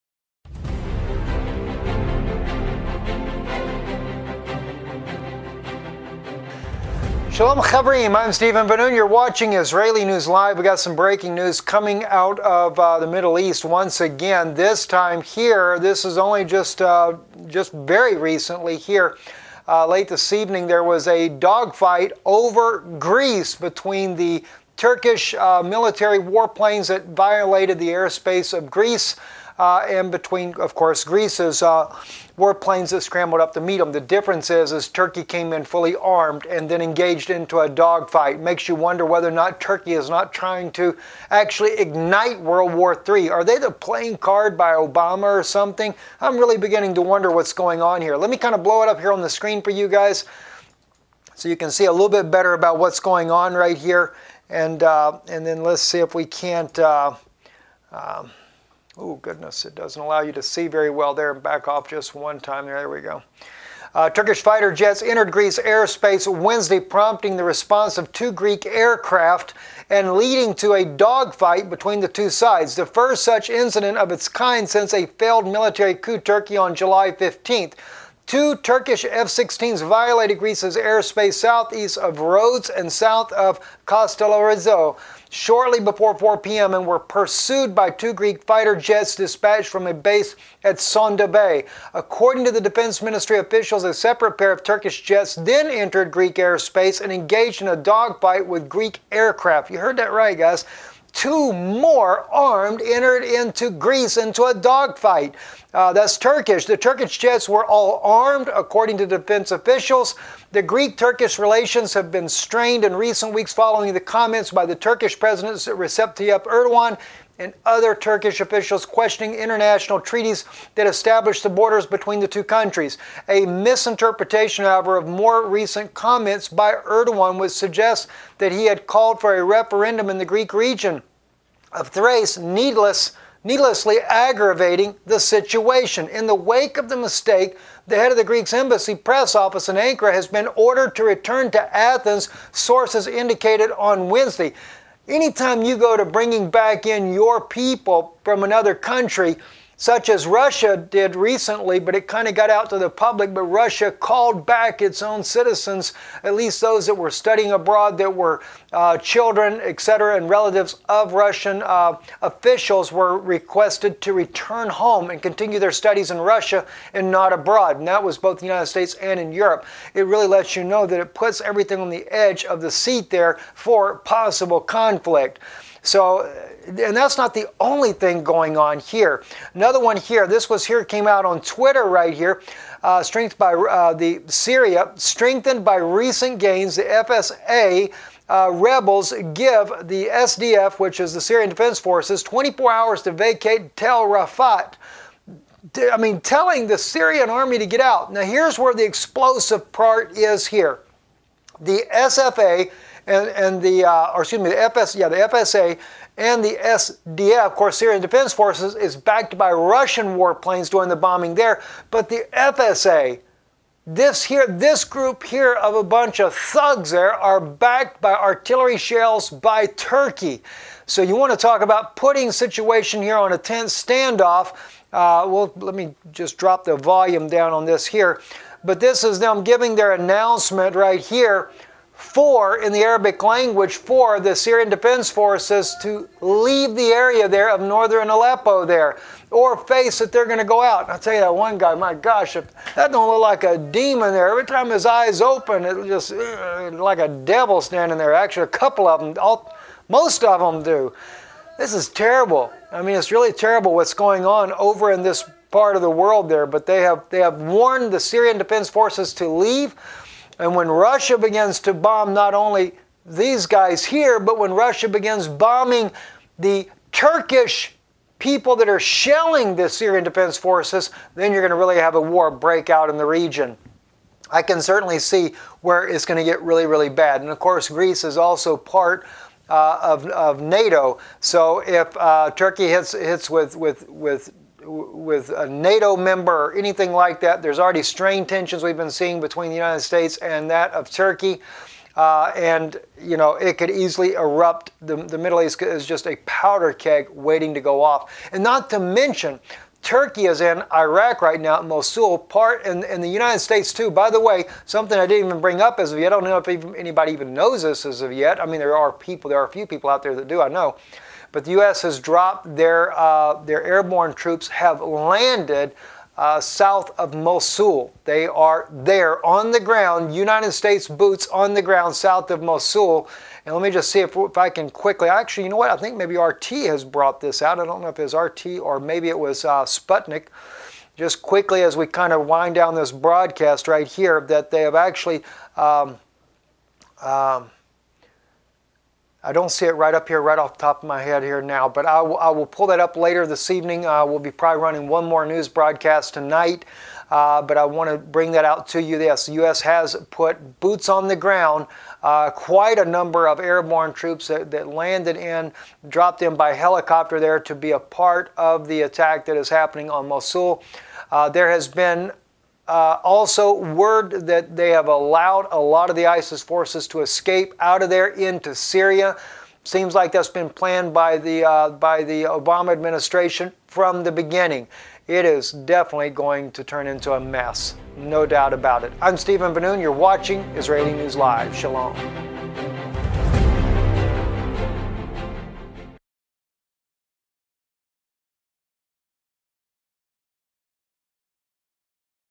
Turkey Instigating War With Greece
Turkey's latest provocations is clearly Instigating a War With Greece. As we watch the tensions building everywhere in the middle east you cannot help but wonder what Turkey is up to. A constant poking and prodding at all its neighbors and even its allies. the armed Turkish war planes got in a dog fight over the Aegean Sea.
Turkey